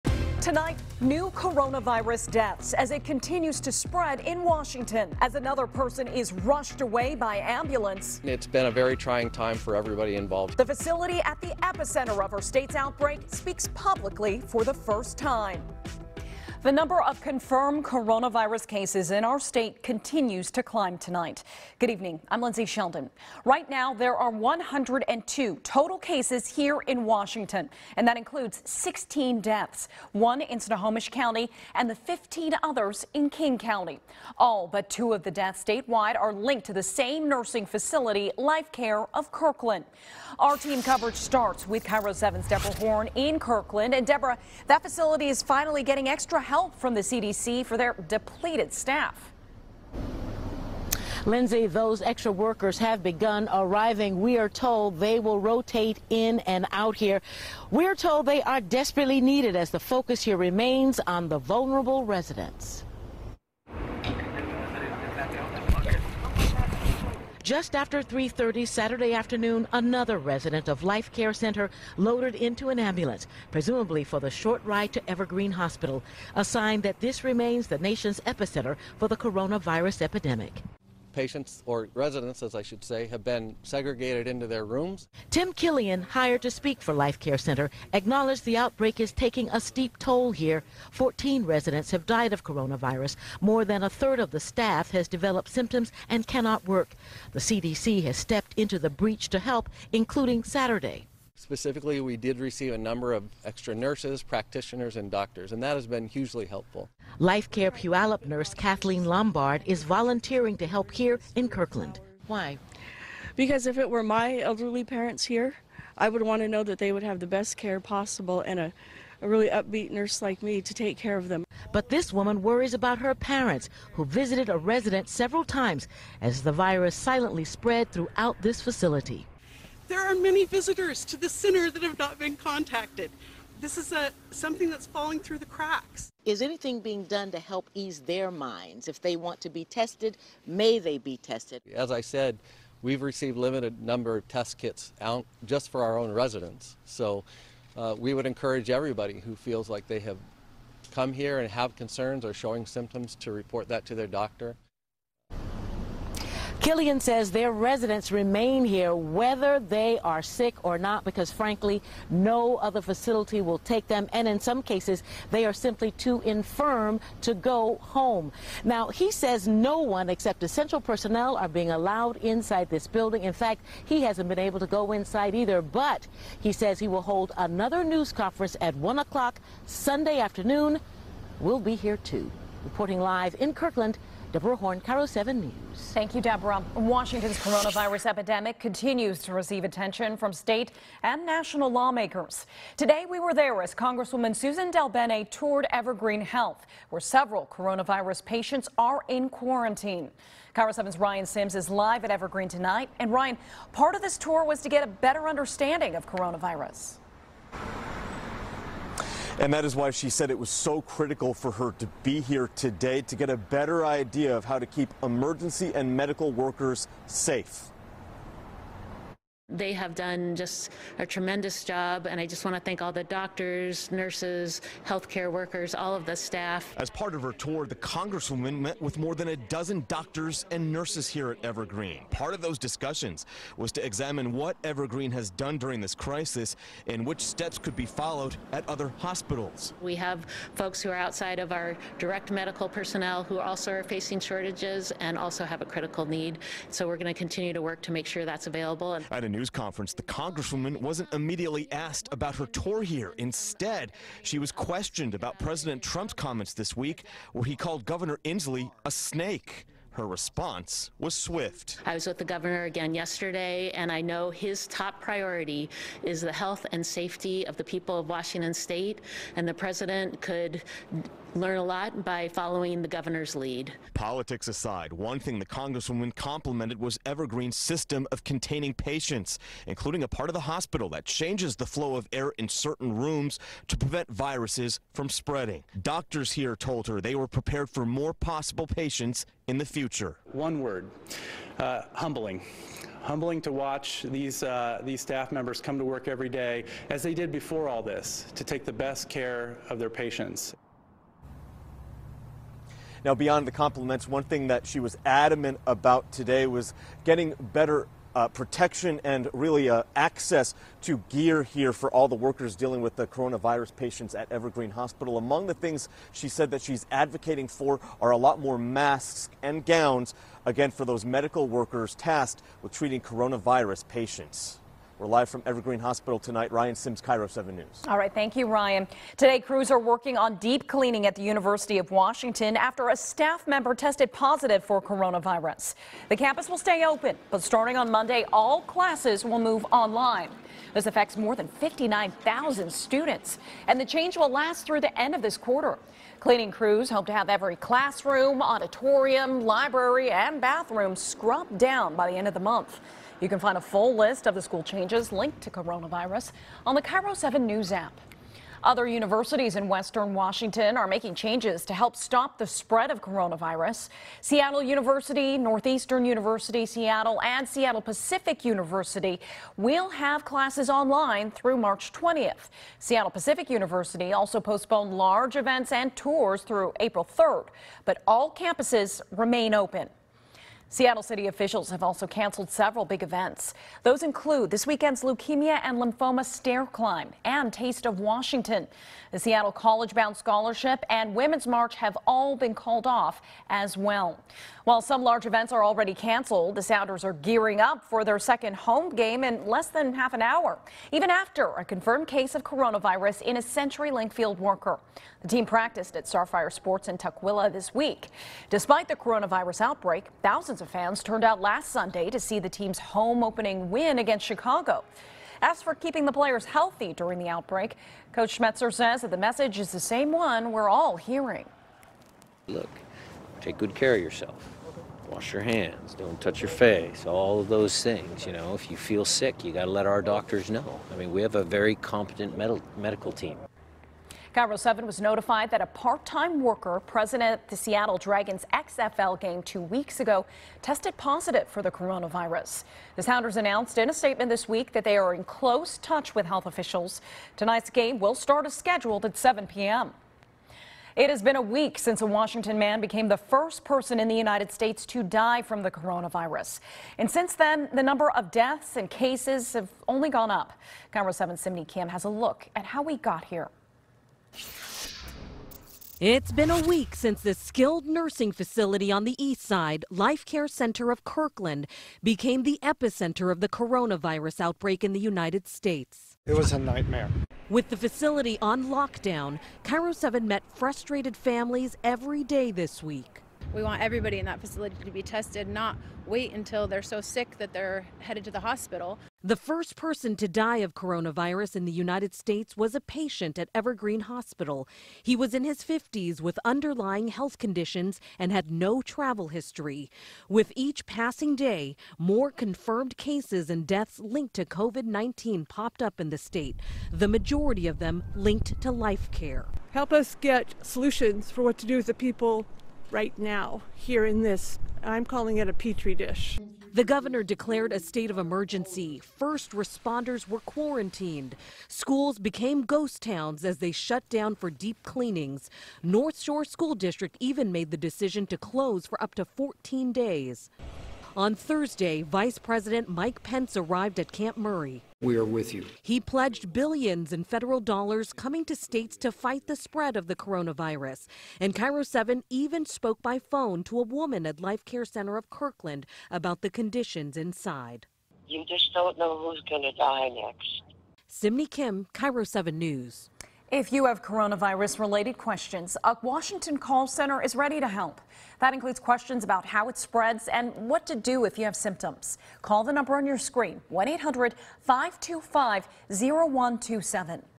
VIDEO: Coronavirus death toll rises in Washington state
There are more than 100 cases, which include 16 deaths in Washington state.